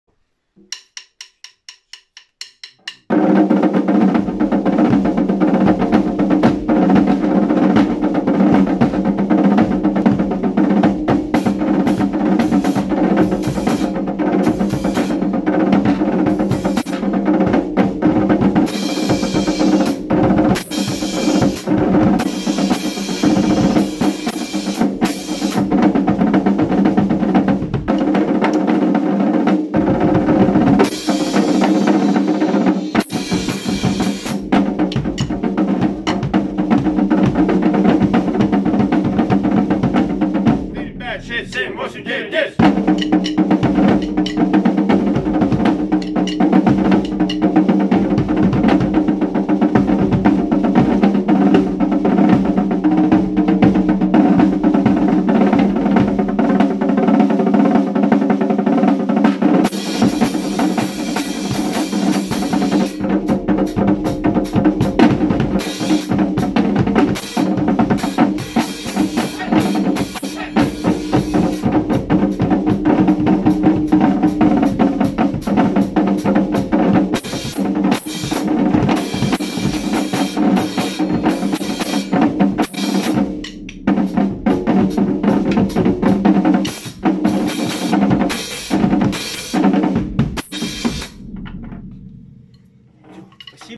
Russian Rudimental Drummers Sky Rhythm
Marching drum group Sky Rhythm-rehearsal without snare system on snare drum

Majestic marching snare drum Endeavor

Sabian pair cymbals B8

Majestic marching bass drum Endeavor